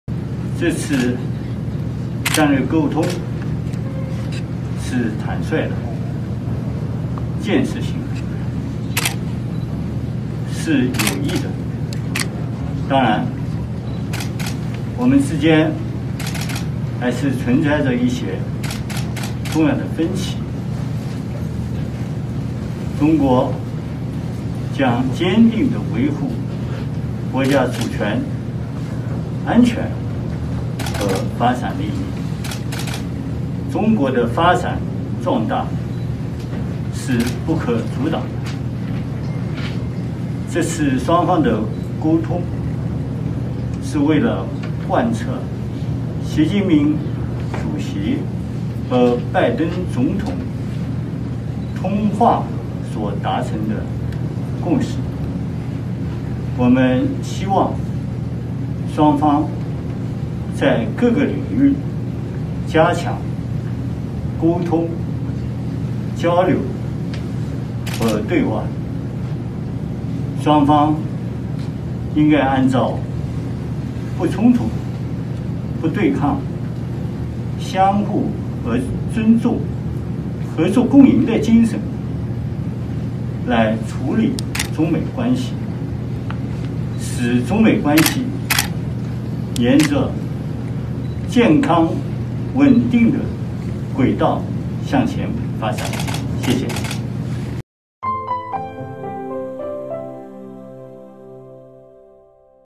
杨洁篪：中国的发展壮大是不可阻挡的
3月18日至19日，中美高层战略对话在安克雷奇举行。对话结束后，中共中央政治局委员、中央外事工作委员会办公室主任杨洁篪、国务委员兼外长王毅接受了媒体采访。
        杨洁篪表示，这次对话是有益的，有利于增进相互了解。双方在一些问题上仍存在重要分歧。中国将坚定维护国家主权、安全和发展利益。中国的发展壮大是不可阻挡的。这次双方的沟通是为了贯彻习近平主席和拜登总统通话所达成的共识。我们希望双方在各个领域加强沟通、交流和对话。双方应按照不冲突不对抗，相互尊重、合作共赢的原则处理中美关系，使中美关系沿着健康、稳定轨道向前发展。（记者 陈孟统 刘关关 制作 乐小敏）